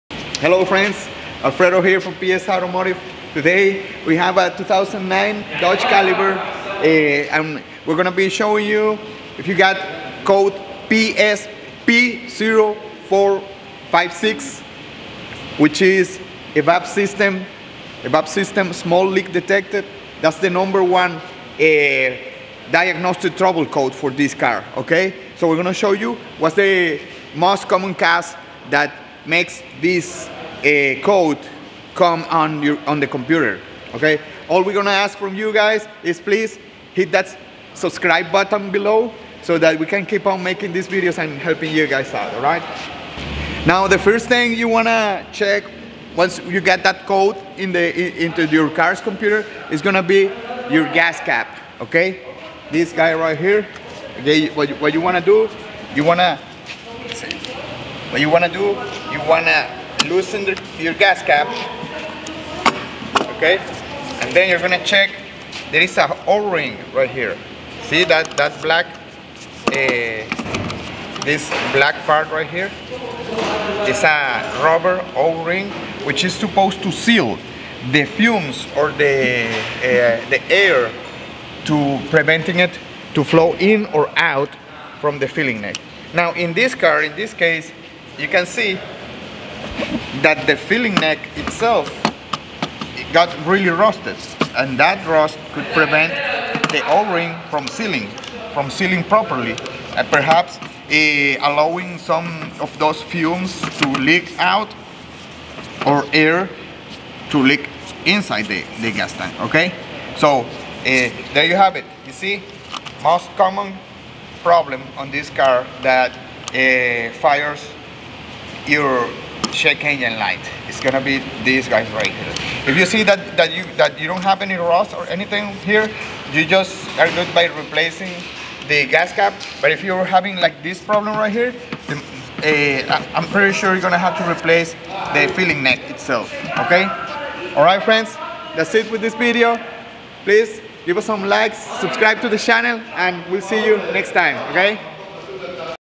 2009 Dodge Caliber P0456 EVAP System Small Leak Fix
2009 dodge caliber P0456 EVAP System Small Leak Fix

In this video Alfredo Perez is going to take about the error code P0456 (Small Leak) on a 2009 Dodge Caliber. The solution can be replacing the gas cap or the entire filling neck. If you have any questions about this car leave it in the comments below.